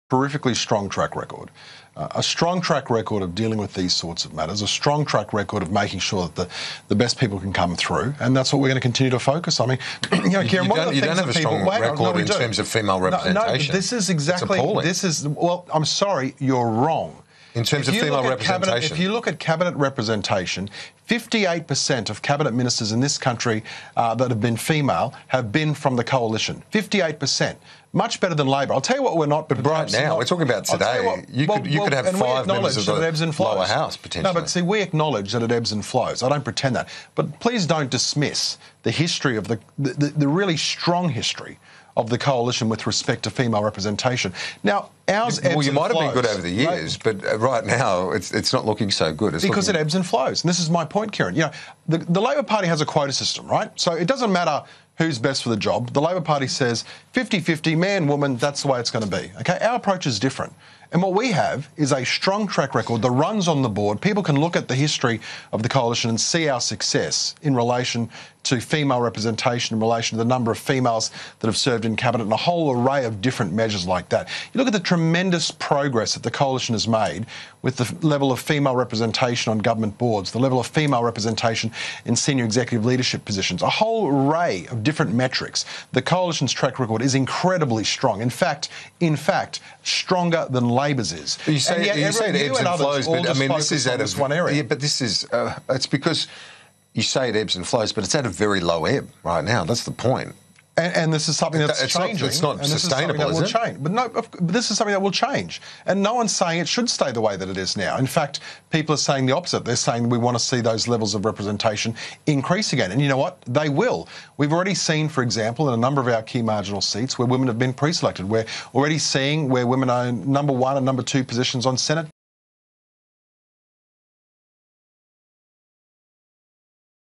Steven Ciobo claims Liberal Party stronger than Labor on female representation
18/09/2018 The Liberal Party has 'a strong track record' of female representation and the ratio in the party 'ebbs and flows', says Steven Ciobo.